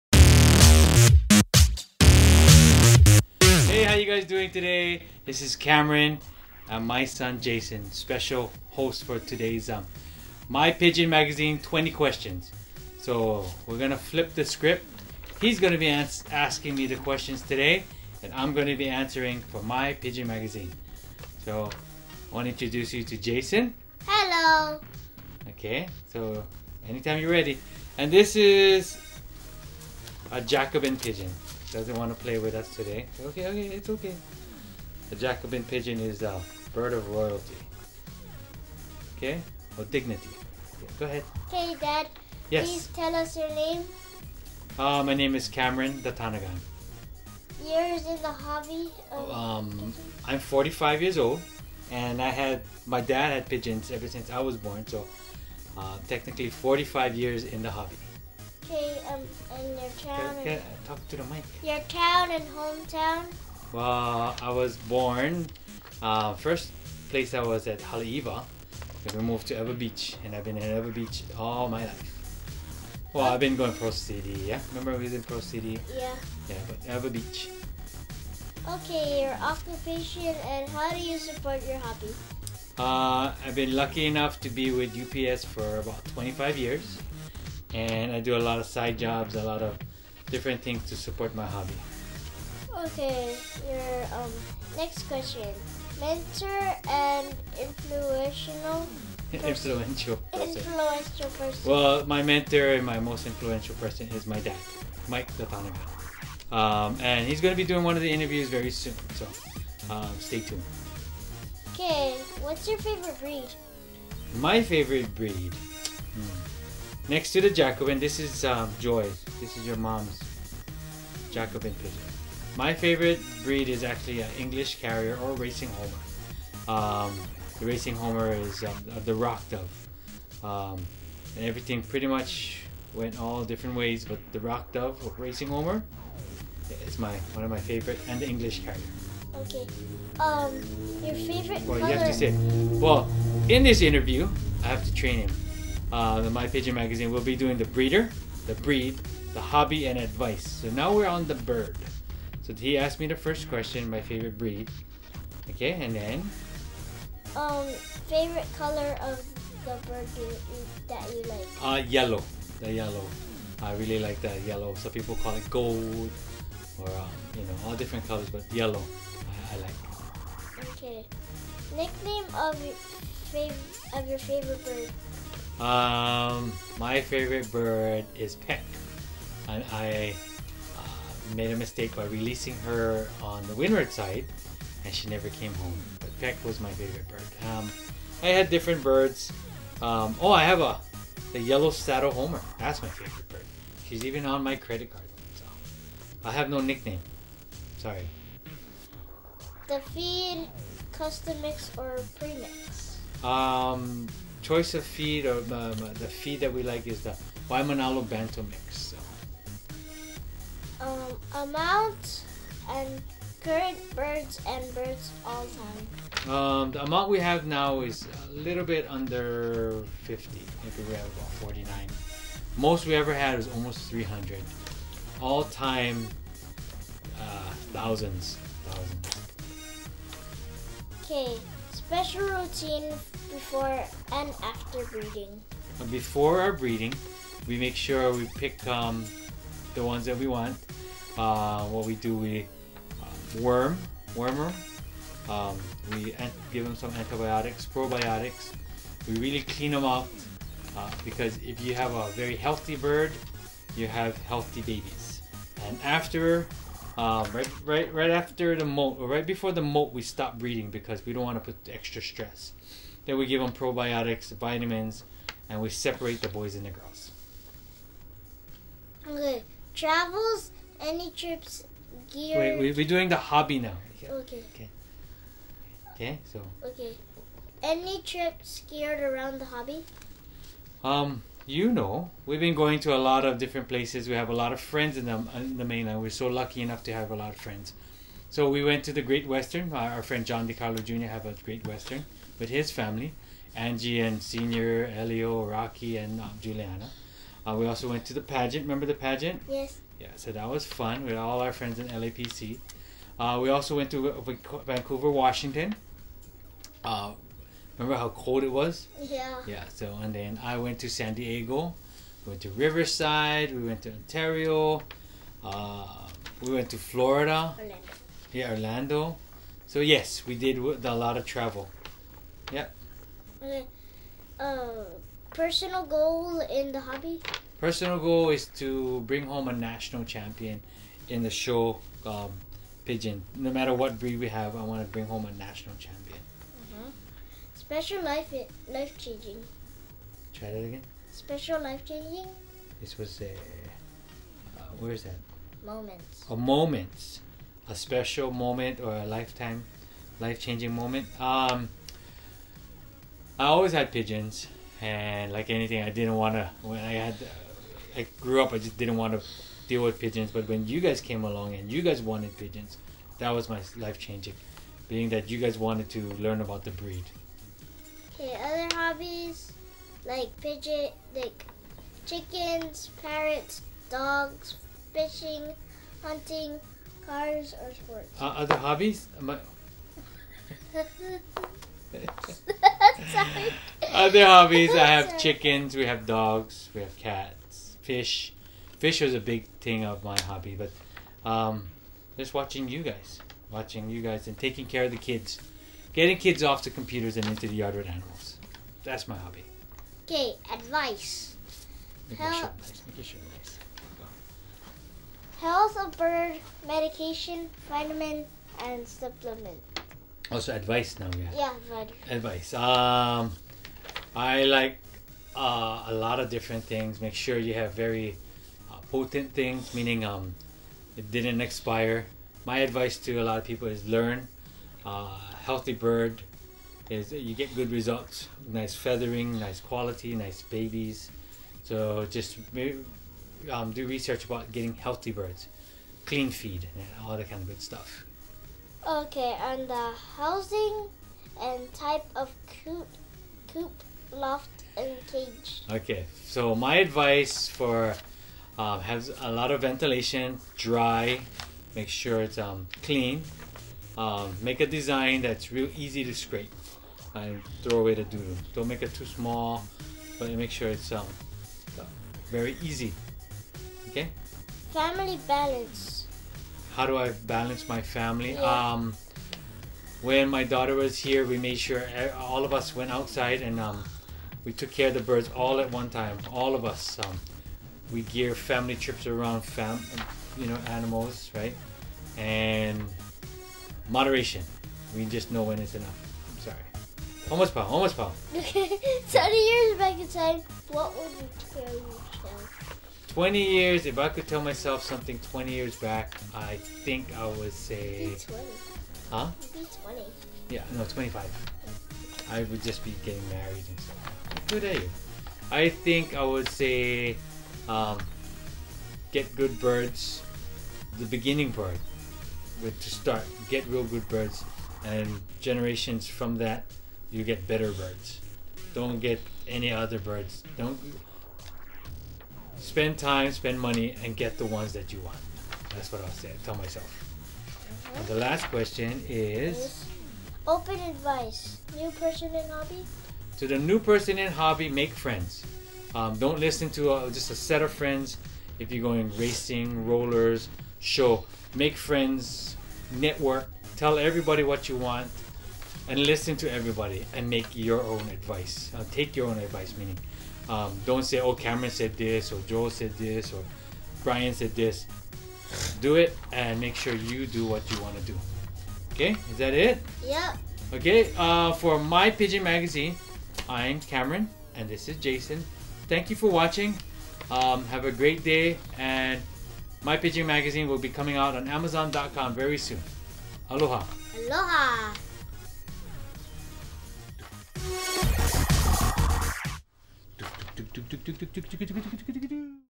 "My Pigeon Magazine" with Cam and Jay
Jayson interviews Cameron for "My Pigeon Magazine"

Breeder, Breed, Hobby and Advice